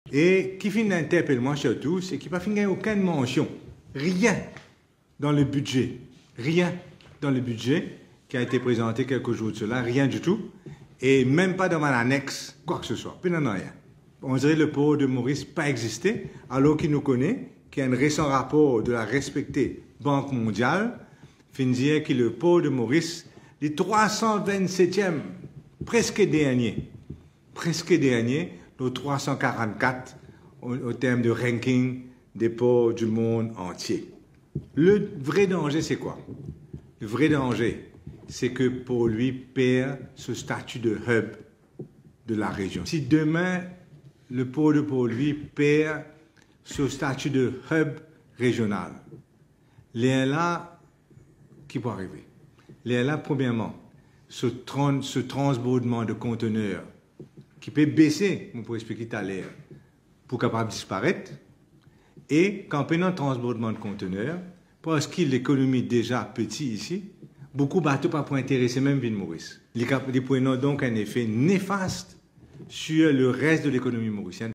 Port-Louis 327e sur le Container Port Performance Index : « Si nous perdons notre statut, cela va...
Port-Louis 327e sur le Container Port Performance Index : « Si nous perdons notre statut, cela va nuire à notre économie », déplore Xavier-Luc Duval